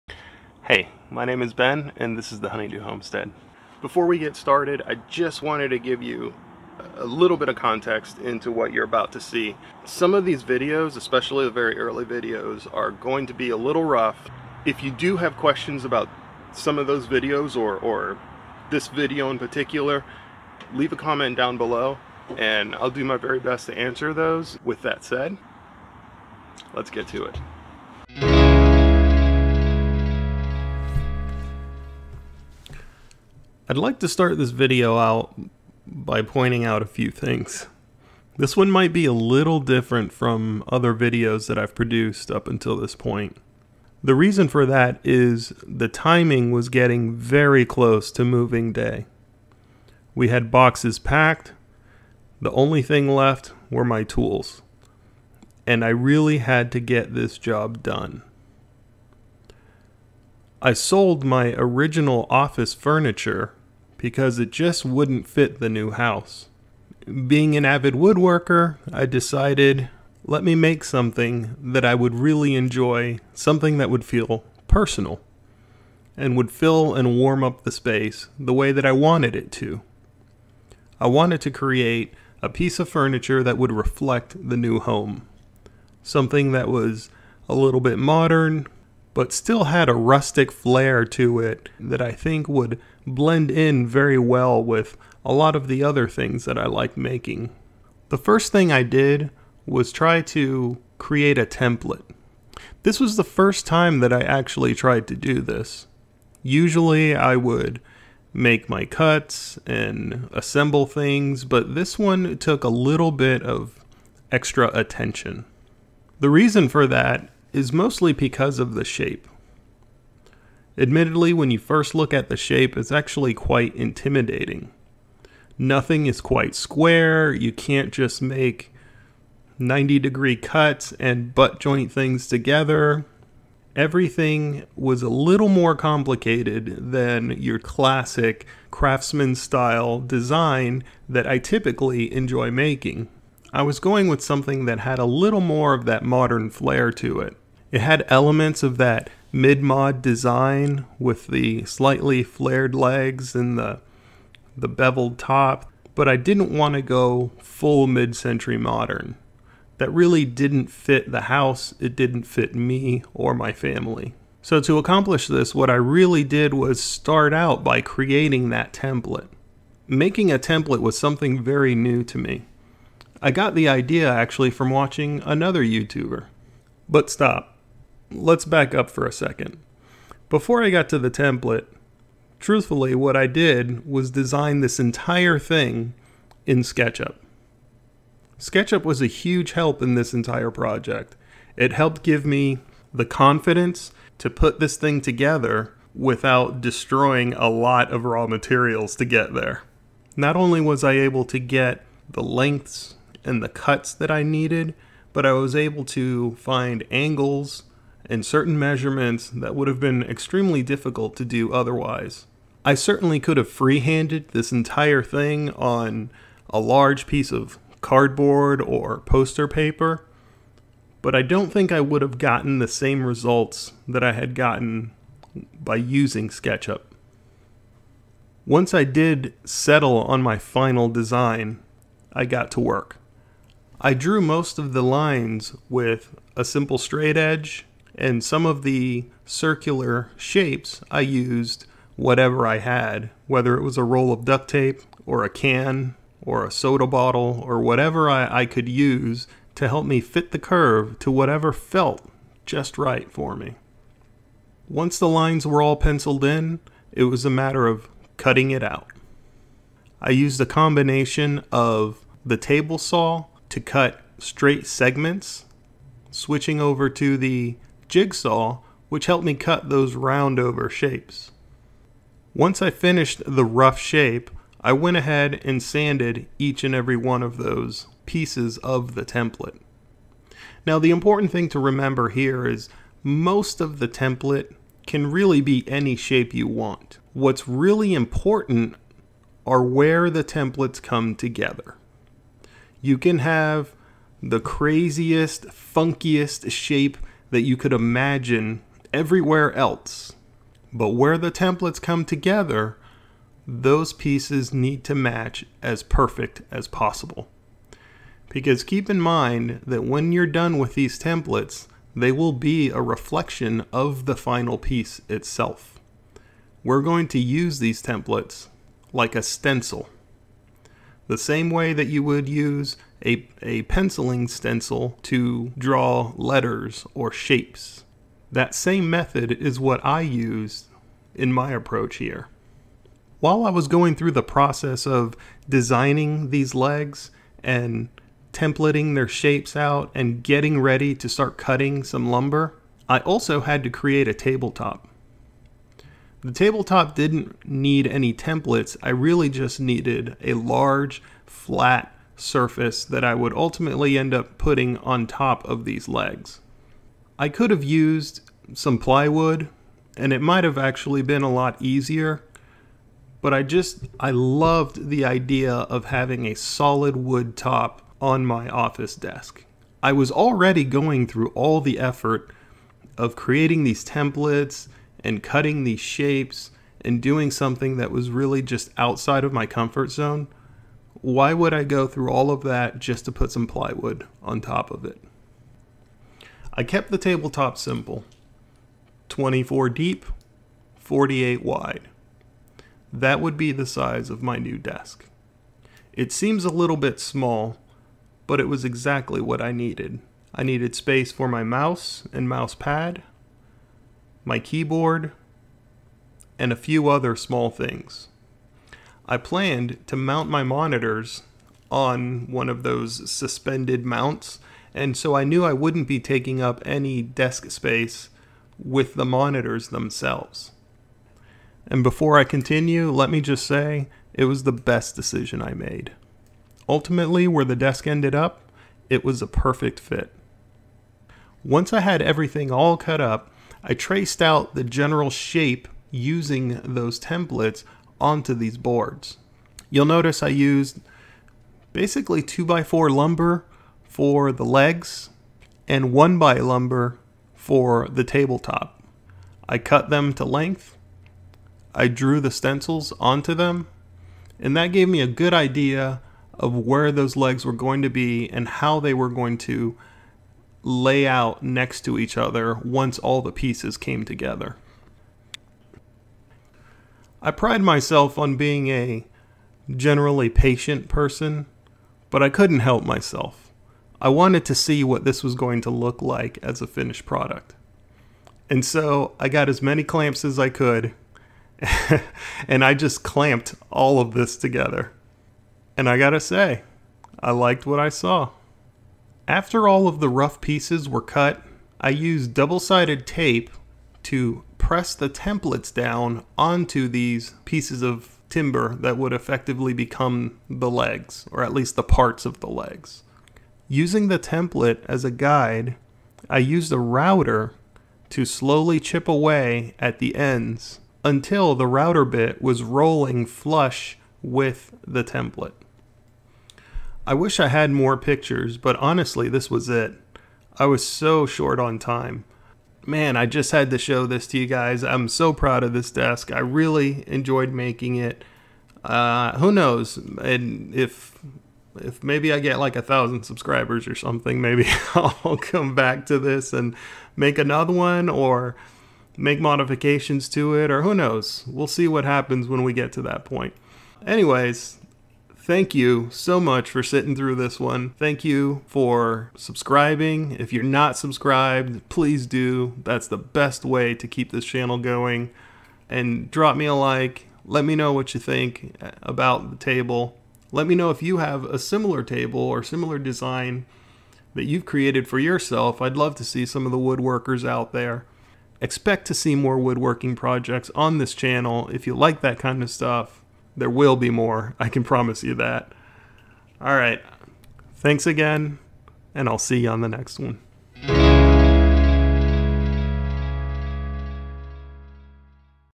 A Mid-century Modern Inspired Office Desk with Rustic Flare
I sold my particle board office furniture, and built a desk that was a reflection of me; a little bit country, a little bit of rock & roll.

I used common pine boards and construction lumber to build a desk that I'd be proud to sell in a shop, if I could part with it. :D  I built a custom desk that no one else owns, and it feels amazing!